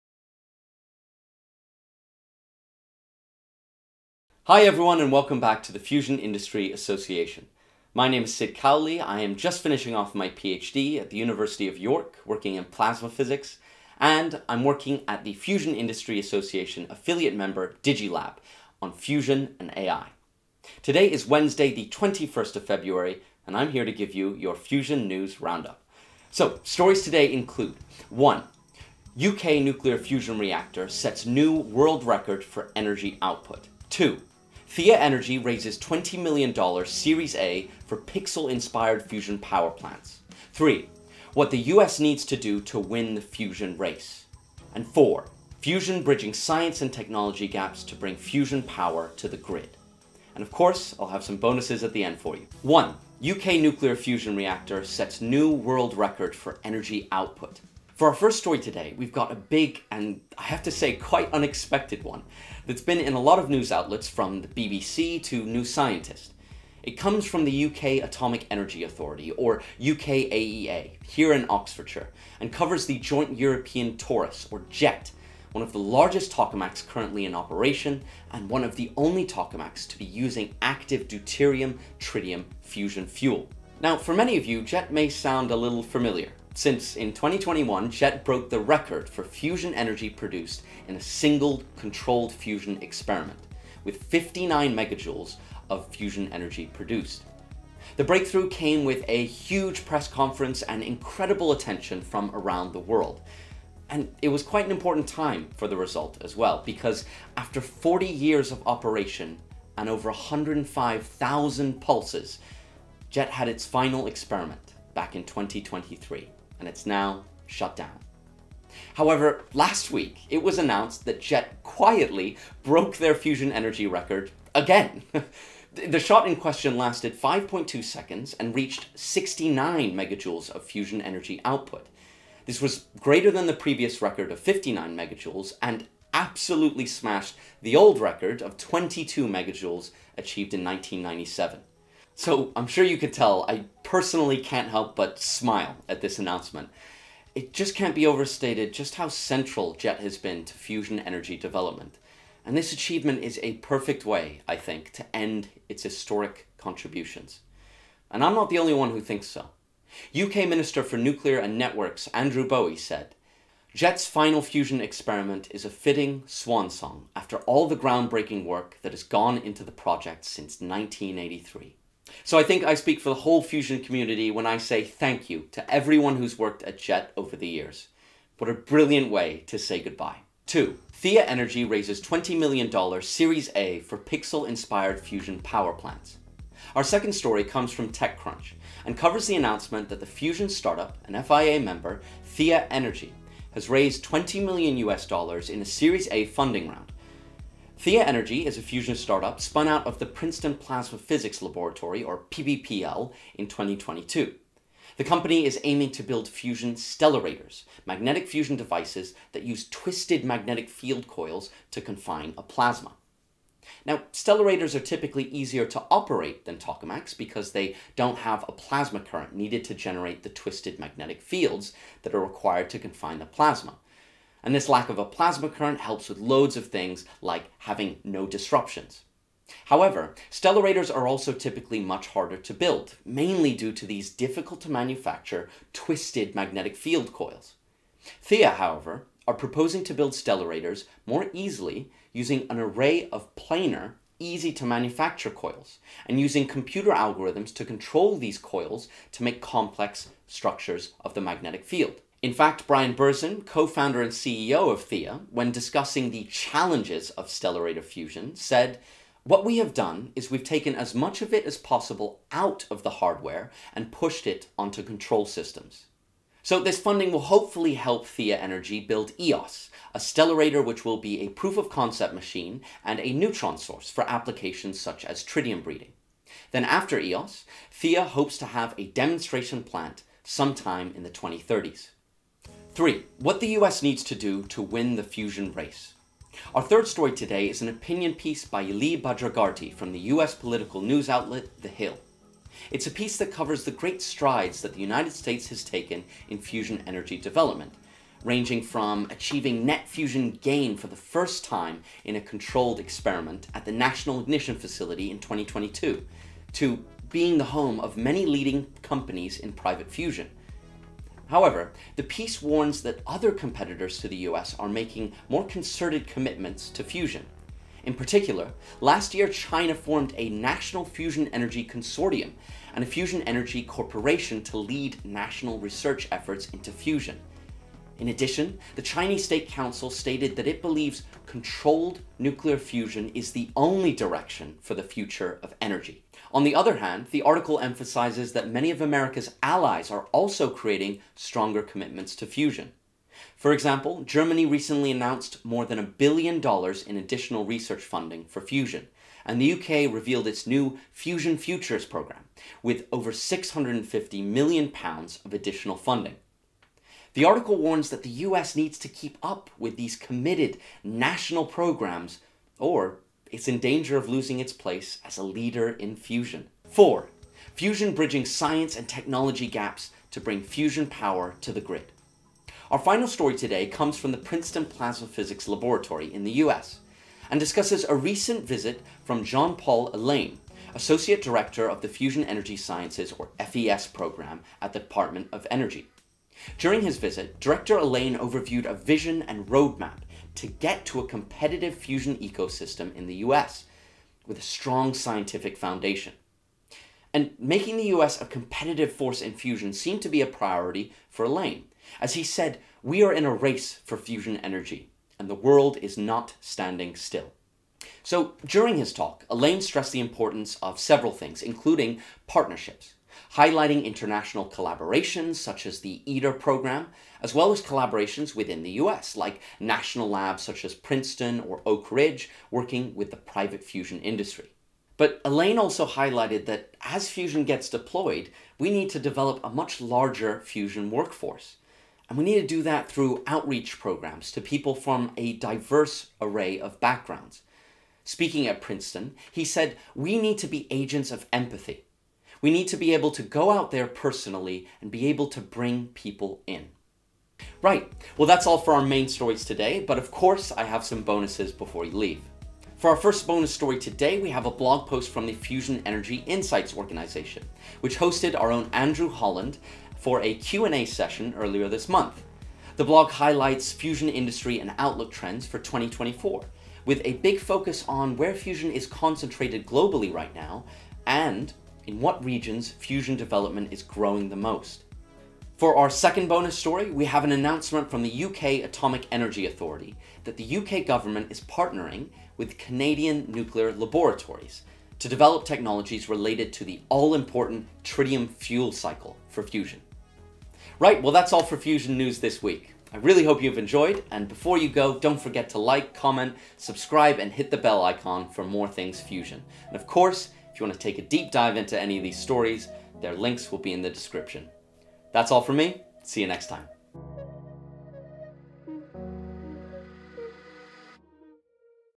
Fusion News, February 21, 2024
Cyd Cowley, PhD student at the University of York and Fusion Solution Consultant at digiLab, summarizes this week's global fusion news update. Links to all the stories mentioned are included below.

1. UK nuclear fusion reactor sets new world record for energy output

2. Thea Energy raises $20M Series A for pixel-inspired fusion power plants

3. What the US needs to do to win the fusion race

4. Fusion Bridging science and technology gaps to bring fusion power to the grid